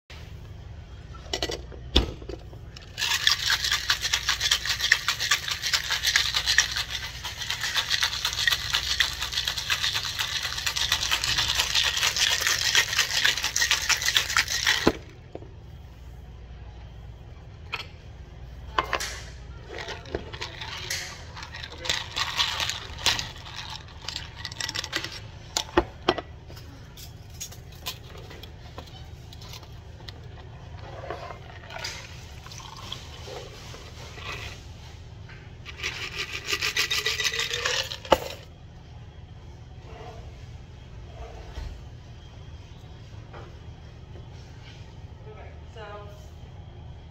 ASMR cocktail shaking and pouring by real bartender
unintentional ASMR of a real bartender shaking and pouring a cocktail in texas